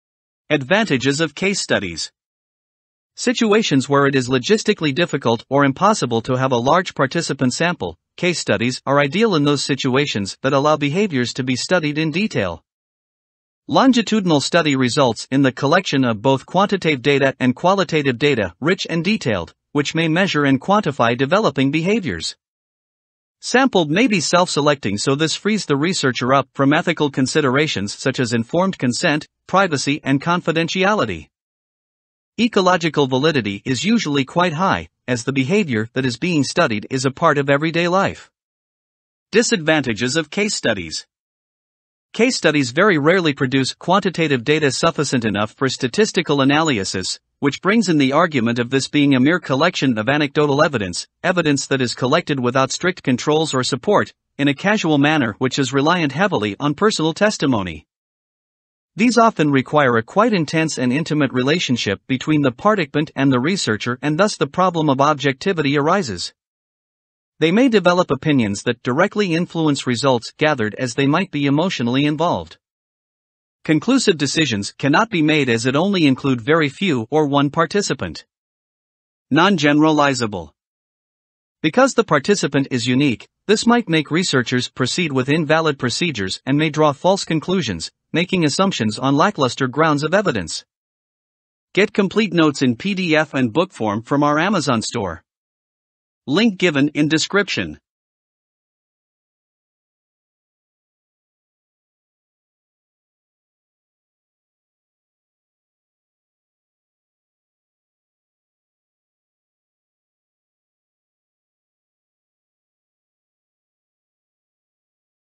Advantages and Disadvantages of Case Studies | A Level Notes Psychology 9990
Situations where it is logistically difficult or impossible to have a large participant sample – case studies are ideal In those situations that allow behaviours to be studied in detail.
Longitudinal study results in the collection of both quantitaive data and qualitative data (rich and detailed), which may measure and quantify developing behaviours.
Sample may be self-selecting so this frees the researcher up from ethical considerations such as informed consent, privacy and confidentiality.
Ecological validity is usually quite high, as the behaviour that is being studied is a part of everyday life.
Disadvantages of Case Studies
Case studies very rarely produce quantitative data sufficent enough for statistical analyisis – which brings in the argument of this being a mere collection of anecdotal evidence (evidence that is collected without strict controls or support, in a casual manner which is reliant heavily on personal testimony.)
These often require a quite intense and intimate relationship between the particpant and the researcher and thus the problem of objectivity arises. They may develop opinions that directly influence results gathered as they might be emotionally involved.
Conclusive decisions cannot be made as it only include very few or one participant. Non-generalizable.
Because the participant is unique, this might make researchers proceed with invalid procedures and may draw false conclusions, making assumptions on lackluster grounds of evidence.